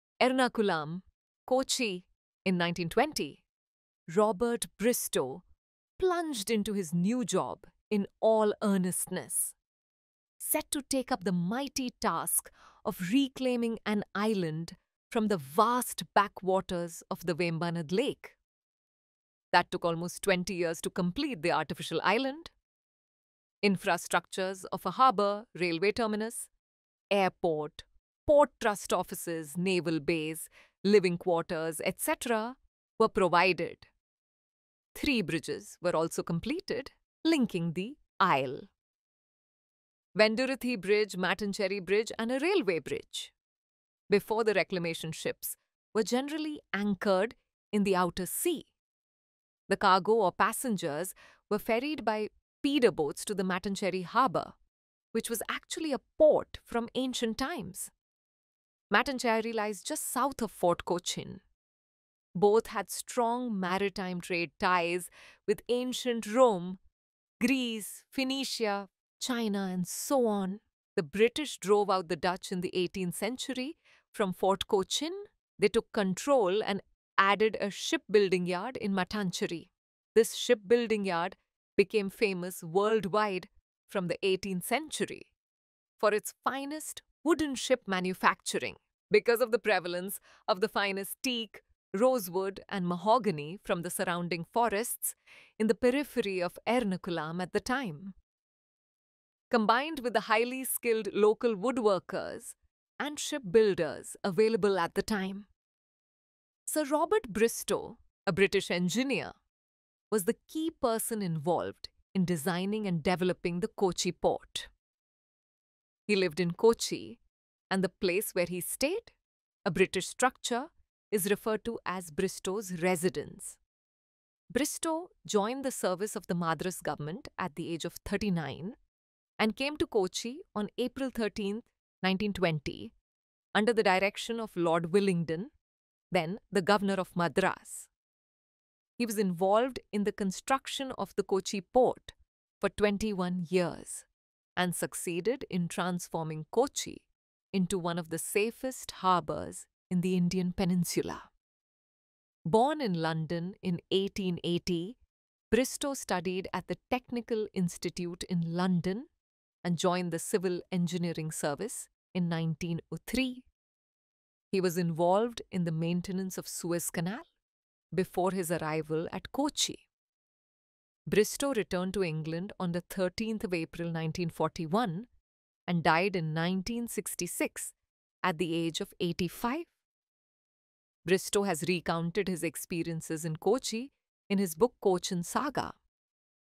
"BRISTOW'S KOCHI" :A PORT OPENED BY A DREAM.
ROBERT BRISTON:A pioneering British harbor engineer best known for transforming Kochi's port infrastructure.